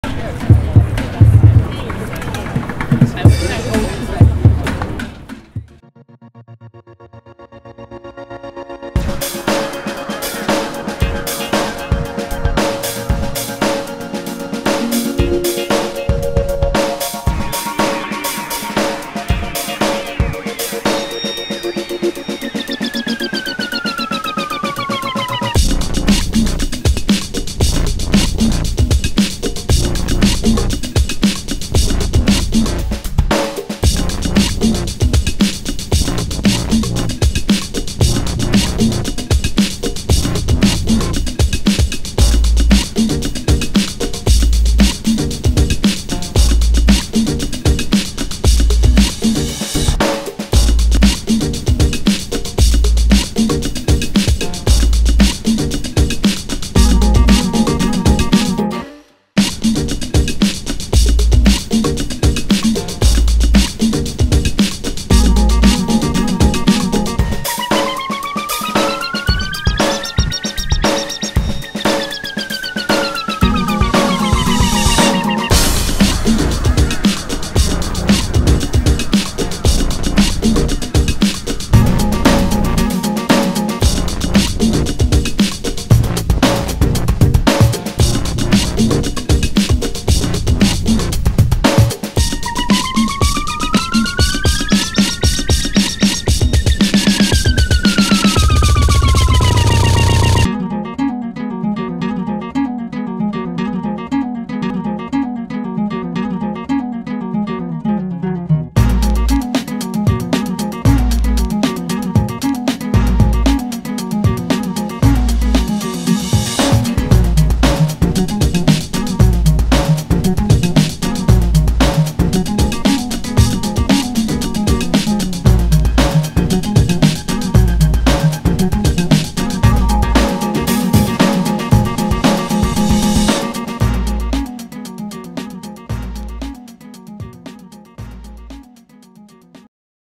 New York : Minute - 10th Avenue - Manhattan, NY
Entering 10 Avenue from West Side Highway in Chelsea neighborhood heading north to Midtown West to 50th Street in Manhattan, New York.

LOCATION : Manhattan, New York